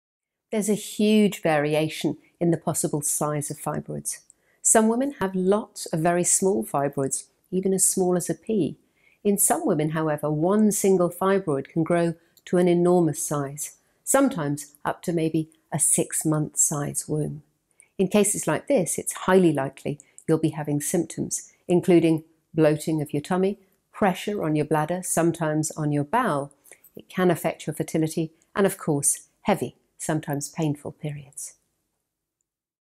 How big can fibroids grow?
Most women who have fibroids are not aware that they have them as they do not have any symptoms. Sometimes one is found during a routine examination by a doctor or by chance during a scan which you may have for another reason.